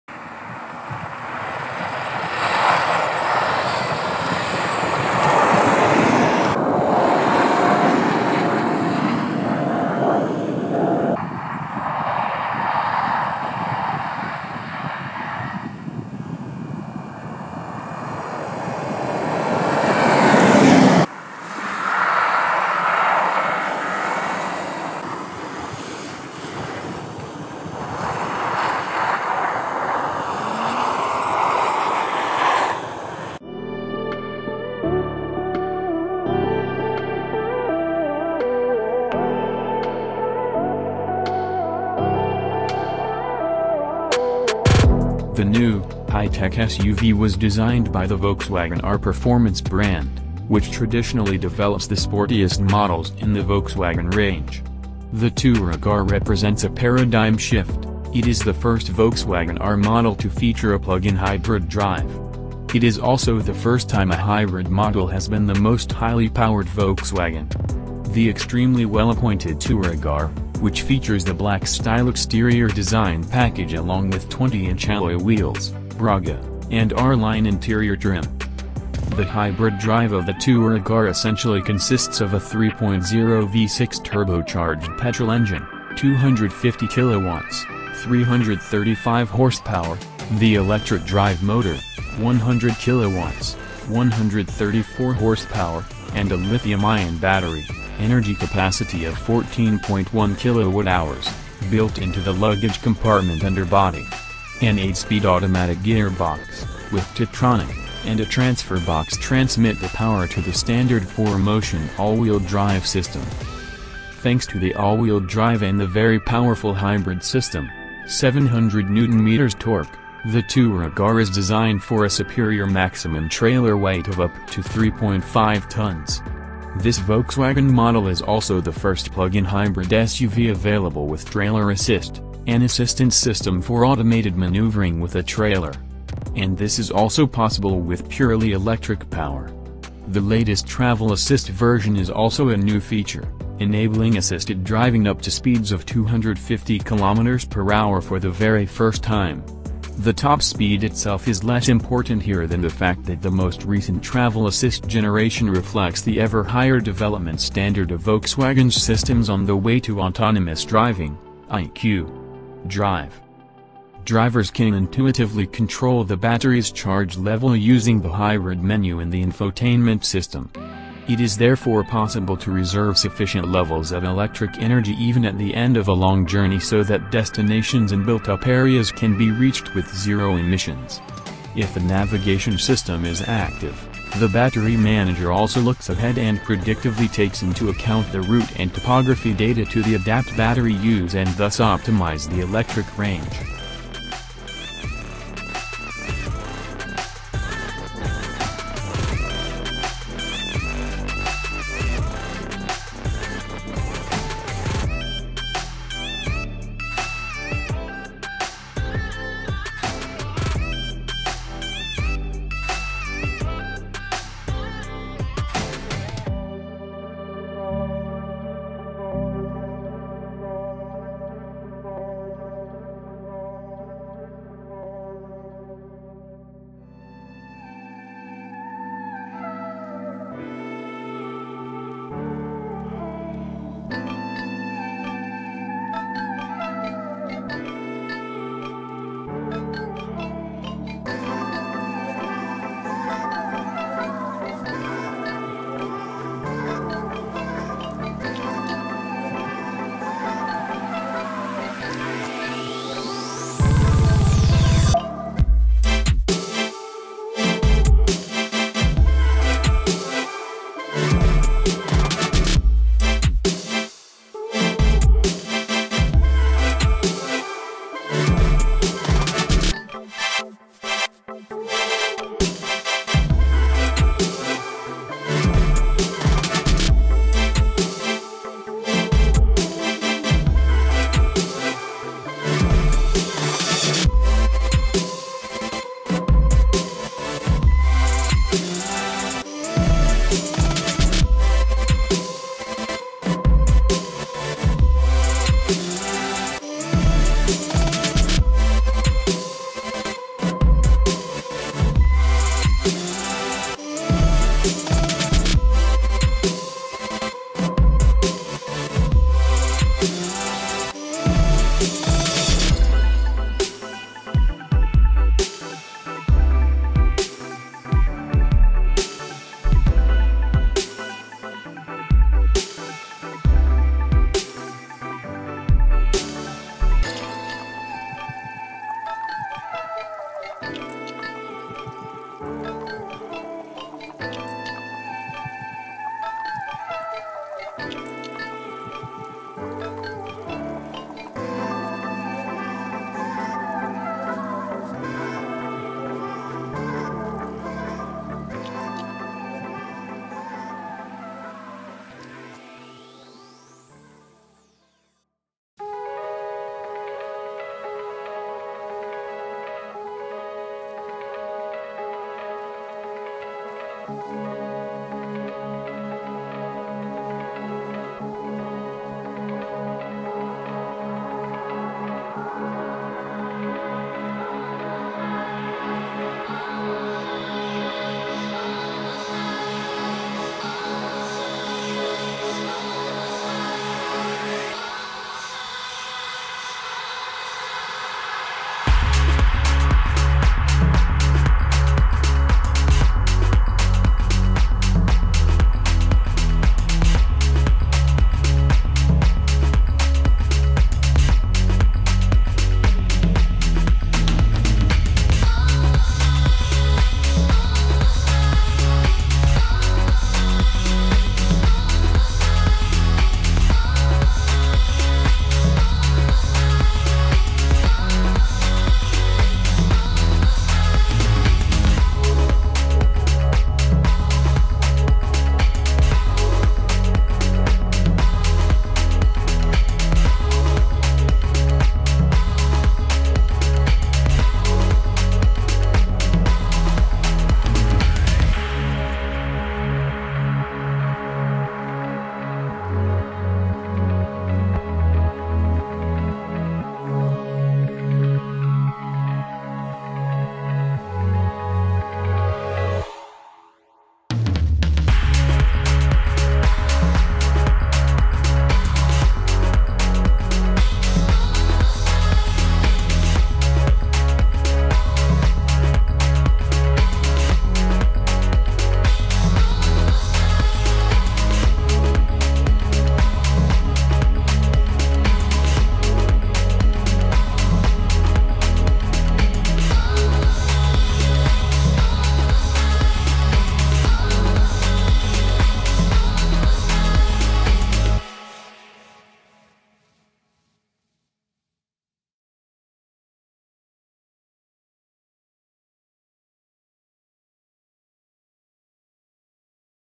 2021 Volkswagen Touareg R - Plug-In Hybrid Flagship SUV
2021 Volkswagen Touareg R - Driving, Exterior & Interior Design

The hybrid drive of the Touareg R essentially consists of a 2,995 cm3 V6 turbocharged petrol engine (250 kW/340 PS), the electric drive motor (100kW/136 PS) and a lithium-ion battery (energy capacity of 14.1 kWh) built into the luggage compartment underbody. An 8-speed automatic gearbox (with Tiptronic) and a transfer box transmit the power to the standard 4MOTION all-wheel drive system.

The new, high-tech SUV was designed by the Volkswagen R performance brand, which traditionally develops the sportiest models in the Volkswagen range. The Touareg R represents a paradigm shift – it is the first Volkswagen R model to feature a plug-in hybrid drive. It is also the first time a hybrid model has been the most highly-powered Volkswagen. The extremely well appointed Touareg R, which features the Black Style exterior design package along with 20-inch alloy wheels (Braga) and R-Line interior trim.

Thanks to the all-wheel drive and the very powerful hybrid system (700 Nm torque), the Touareg R is designed for a superior maximum trailer weight of up to 3.5 tonnes. This Volkswagen model is also the first plug-in hybrid SUV available with Trailer Assist – an assistance system for automated manoeuvring with a trailer. And this is also possible with purely electric power. The latest Travel Assist version is also a new feature, enabling assisted driving up to speeds of 250 km/h for the very first time. The top speed itself is less important here than the fact that the most recent Travel Assist generation reflects the ever higher development standard of Volkswagen’s systems on the way to autonomous driving (IQ. DRIVE).

Drivers can intuitively control the battery’s charge level using the Hybrid menu in the Infotainment system. It is therefore possible to reserve sufficient levels of electric energy even at the end of a long journey so that destinations in built-up areas can be reached with zero emissions. If the navigation system is active, the battery manager also looks ahead and predictively takes into account the route and topography data to the adapt battery use and thus optimise the electric range.